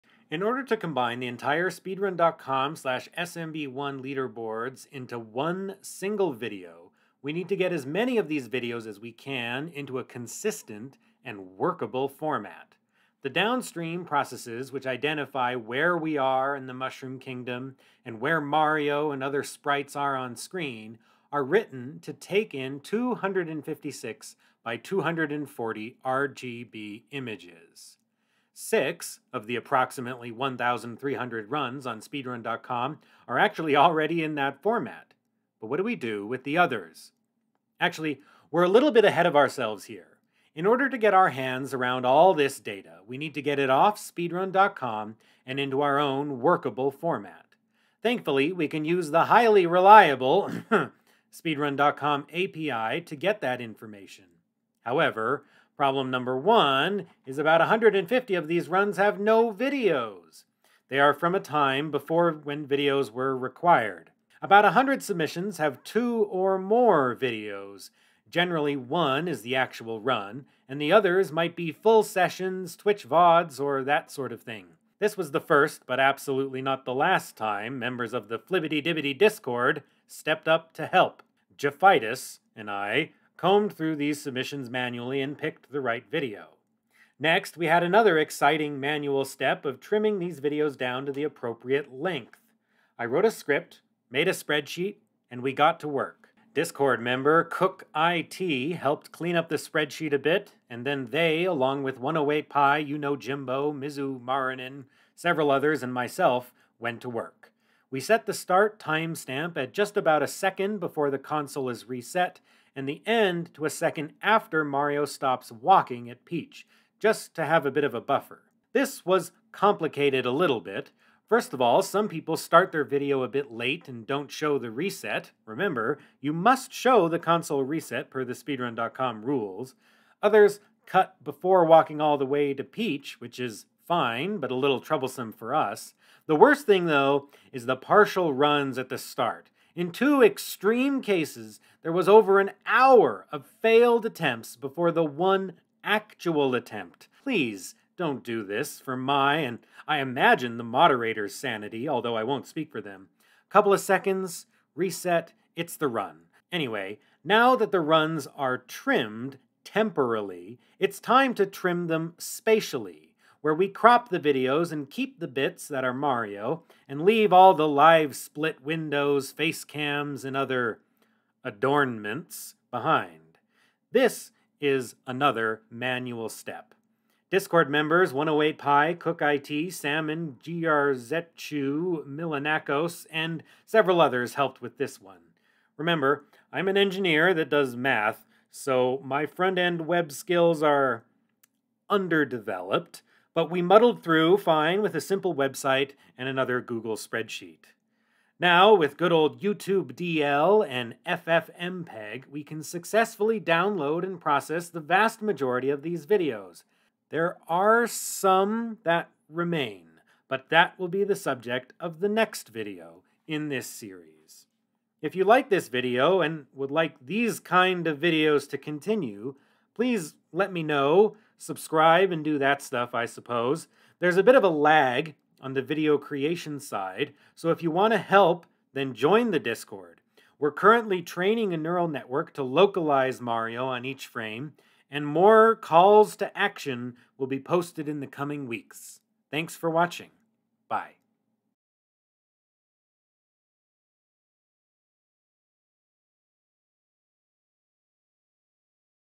Trimming and Cropping Videos - Devlog 1
I don't know what I'm doing with YouTube - but whatever it is I don't particularly want to stick the 'How it's Made' type stuff in with the main 'artsy' videos. If you're here at this video, then you probably care at least a little bit about the math / code / and the actual mechanics of making these kinds of videos / the future of this technology.

I think there are ways I can share the development in a realistic way that works for me, which at least for now is in somewhat unpolished short videos like this one.